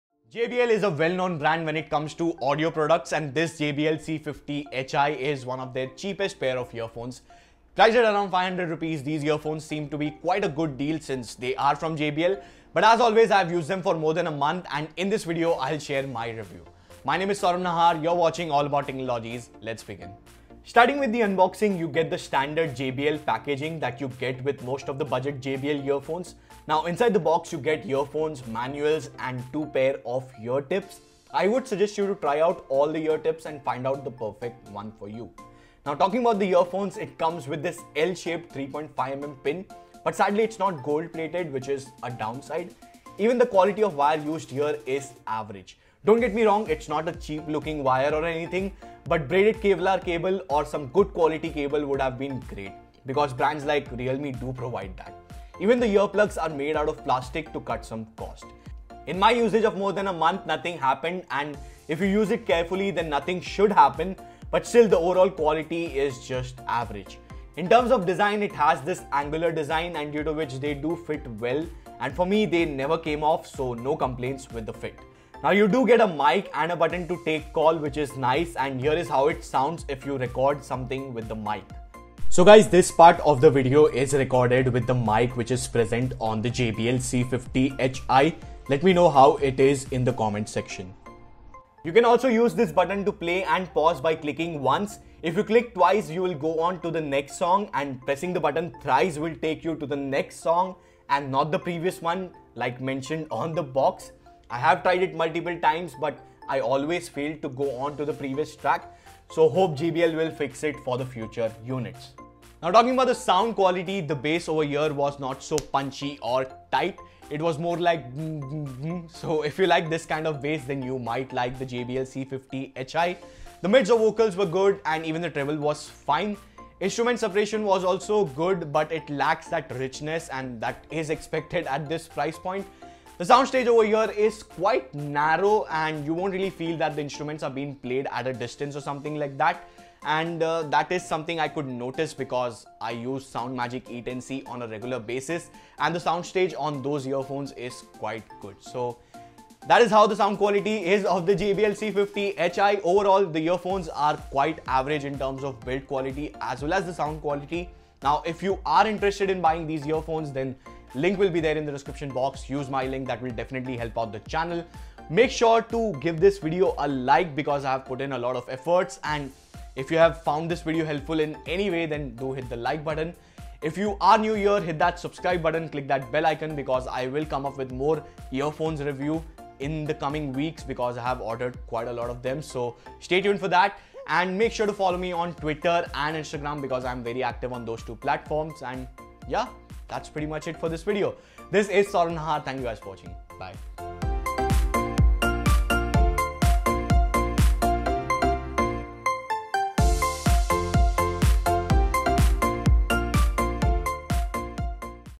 JBL C50Hi Review - These Earphones are nothing but.....
JBL C50Hi is one of the cheapest pair of JBL earphones. You can get them for just Rs. 499 but are they worth it? watch the video😉

Support Us :)